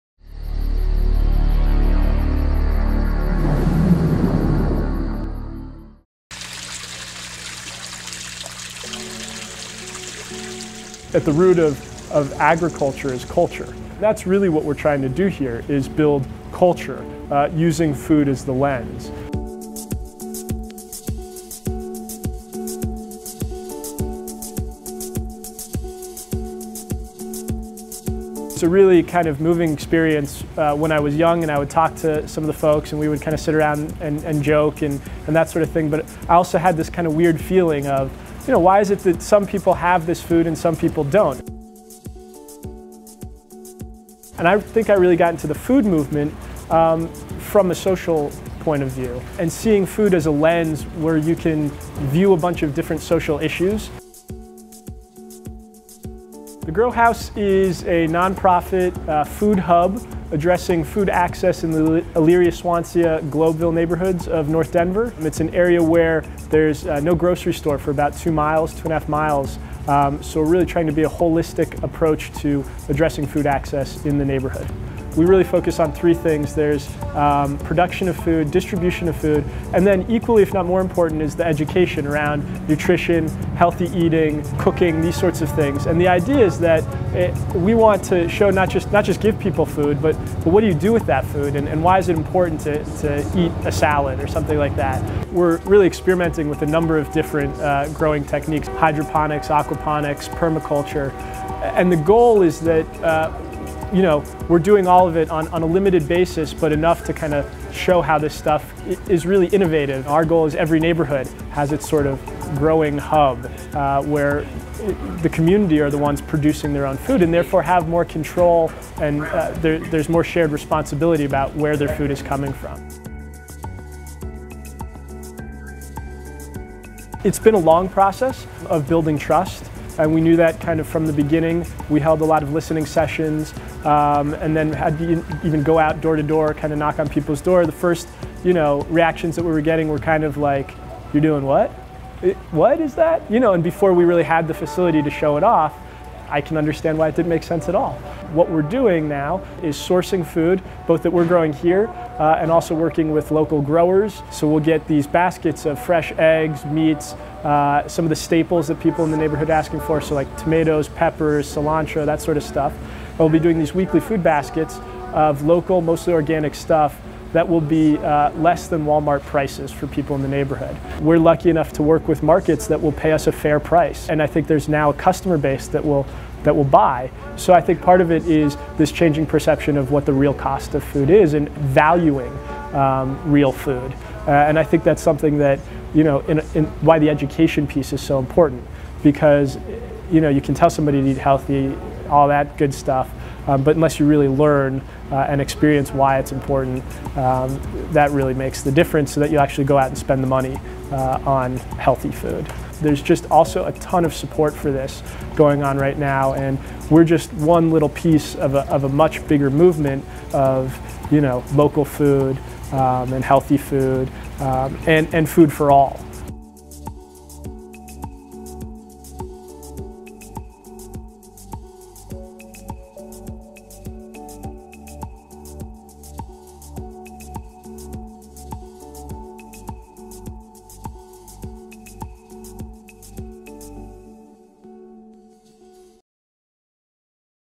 Food For Thought at The Grow Haus
The Grow Haus is an organization working in Denver to help bring healthy food options to the "food deserts" in the area.  The Grow Haus uses aquaponics to effectively grow and sustain diverse  gardens in an urban environment.